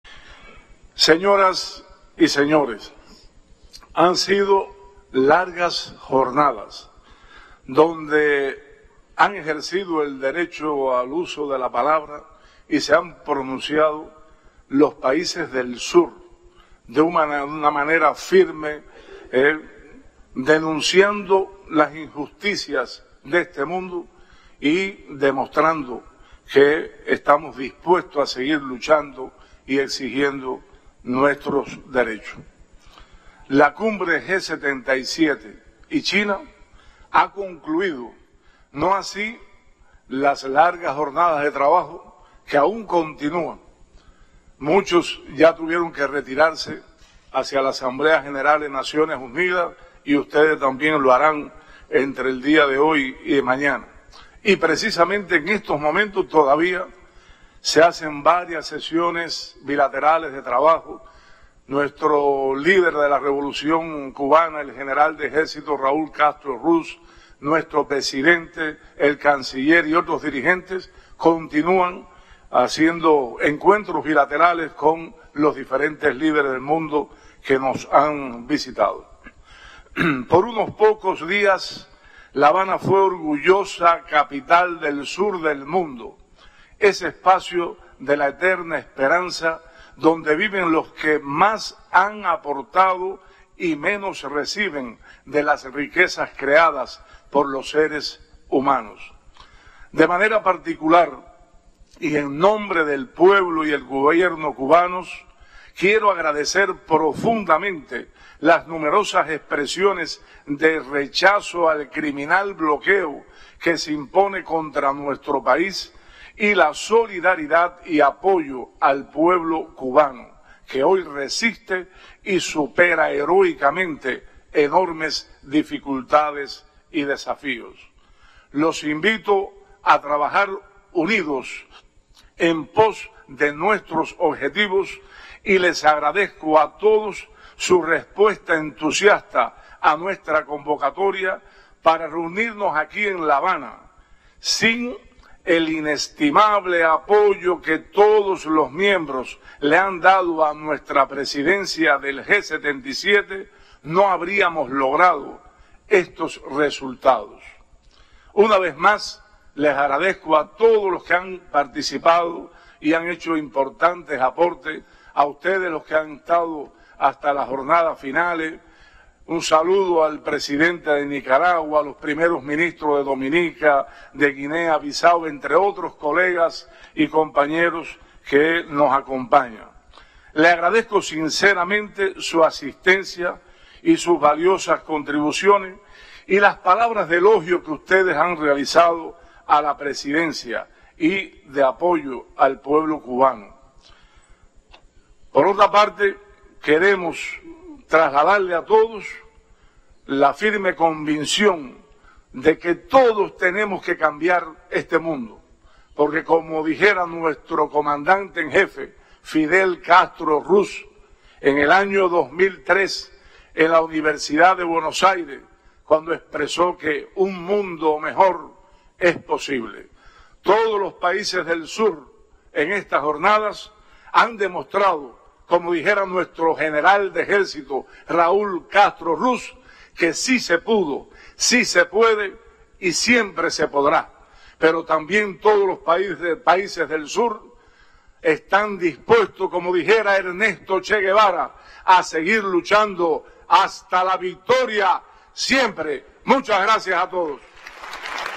Primer ministro de Cuba, Manuel Marrero clausura la Cumbre del Grupo de los 77 y China
El primer ministro de la República de Cuba, Manuel Marrero Cruz clausuró hoy la Cumbre del Grupo de los 77 y China que sesionó en La Habana los días 15 y 16 de septiembre con la participación de más de centenar de delegaciones del mundo.